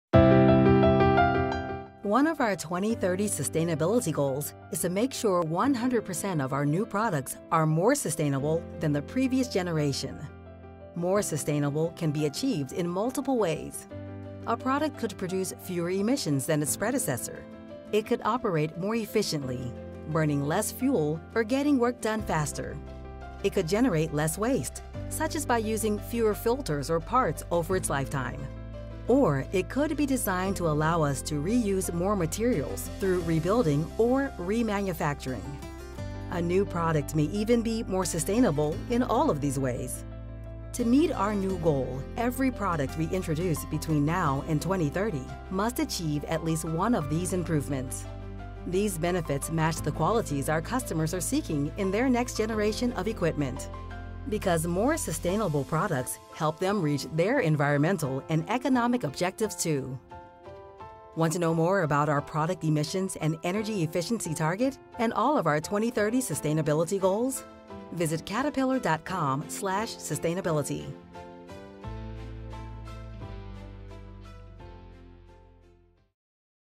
Product Emissions & Energy Efficiency 2030 Sustainability Goal
Learn more about Caterpillar's goal to make 100% of our new products more sustainable than the previous generation through collaborating with customers, reduced waste, improved design for rebuild/remanufacturing, lower emissions or improved efficiency. More information on Sustainability at Caterpillar is available